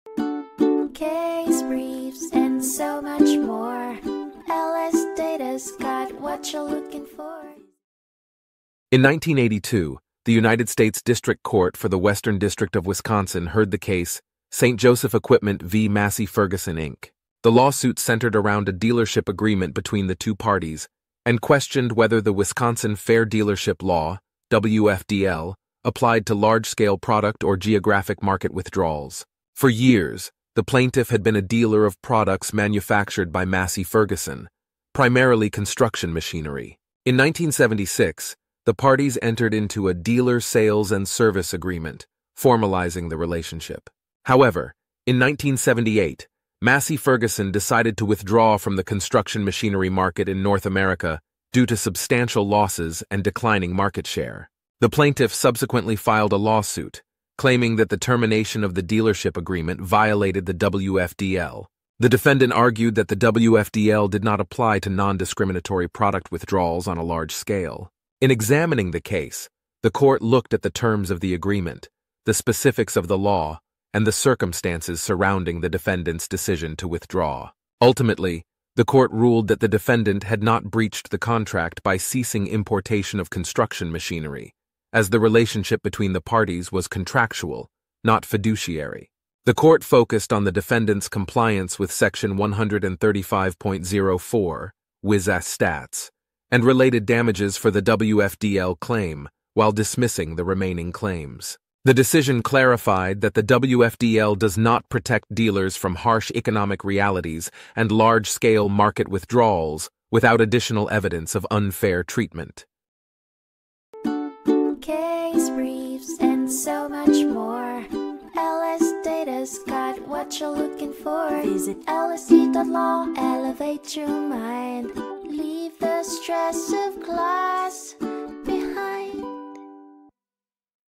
St. Joseph Equipment v. Massey-Ferguson, Inc. (1982) Overview | LSData Case Brief Video Summary
The case involves a plaintiff who alleges that the defendant violated the Wisconsin Fair Dealership Law by withdrawing from the construction machinery market in North America, which effectively terminated the plaintiff's dealership agreement without good cause. The defendant argues that the WFDL does not apply in cases where the grantor undertakes a non-discriminatory withdrawal from a product market on a large geographic scale. The most relevant facts to the court's analysis include the terms of the dealership agreement, the language of the WFDL, and the circumstances surrounding the defendant's decision to withdraw from the market.

St. Joseph Equipment v. Massey-Ferguson, Inc. (1982)
United States District Court for the Western District of Wisconsin
546 F. Supp. 1245